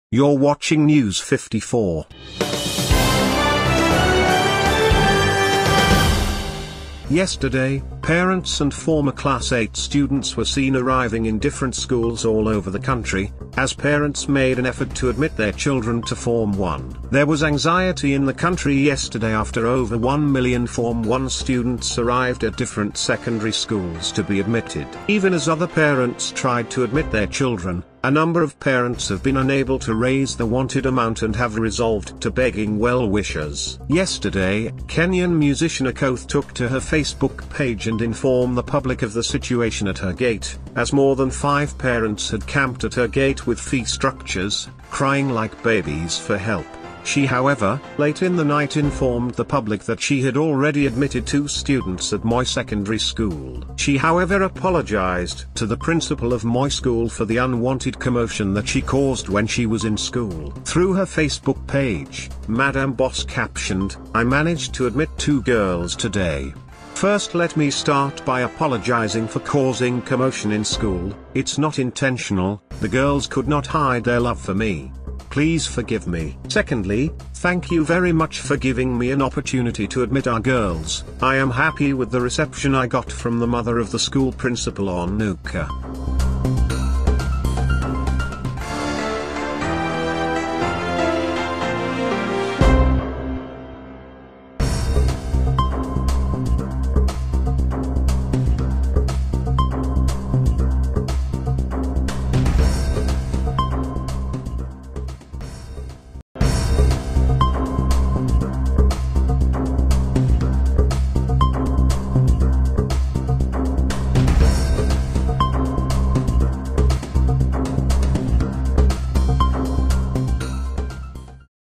Singer AKOTHEE Forced To Apologize After Doing This During Form Ones Admission| News54!
Yesterday, Kenyan musician Akother took to her Facebook page and inform the public of the situation at her gate, as more than five parents had camped at her gate with fee structures, crying like babies for help.

𝗔𝗕𝗢𝗨𝗧 𝗡𝗲𝘄𝘀𝟱𝟰!